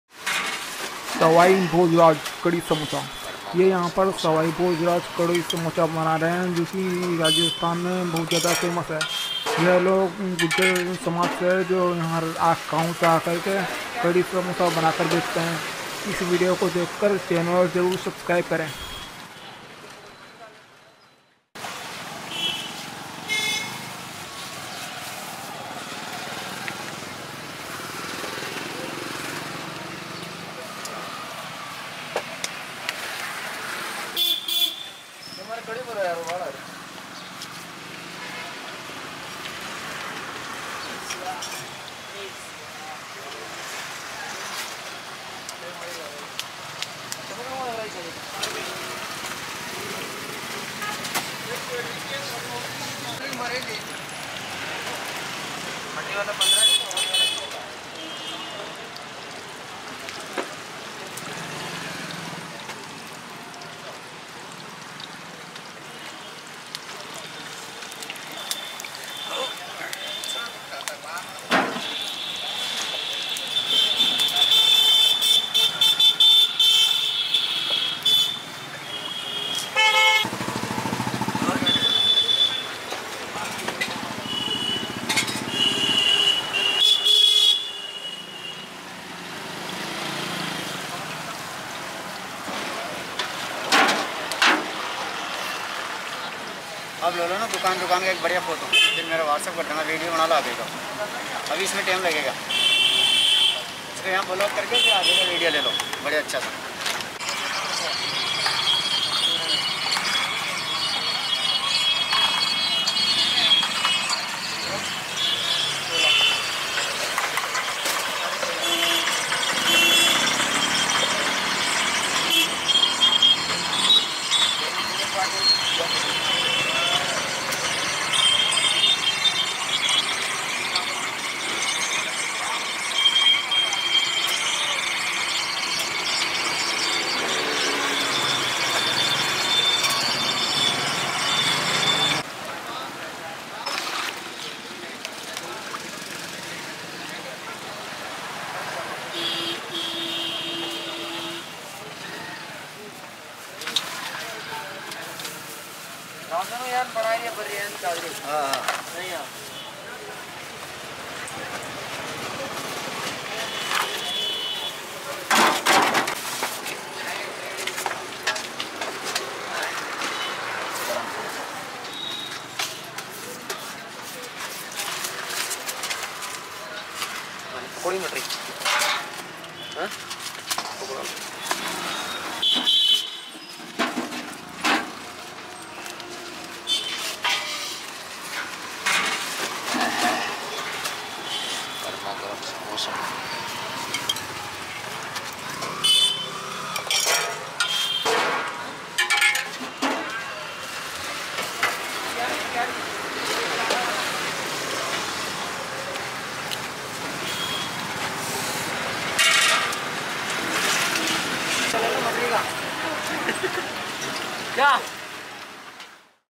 Most famous sawai bhoj kadi samosa of rajasthan - Indian Street Food
I am a deaf guy from Kota, Rajasthan. I record stree food videos and upload it here.